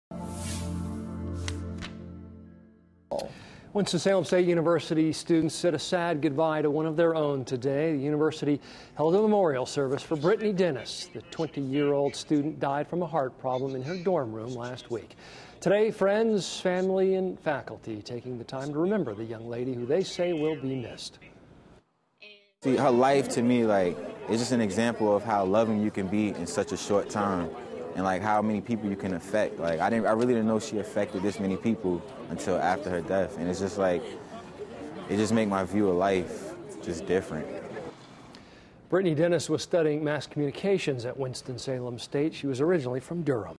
Memorial Held For Student Found Dead In Dorm
A memorial was held on Thursday to remember Winston-Salem State University student Brittany Dennis, who was found dead inside a dorm room last week.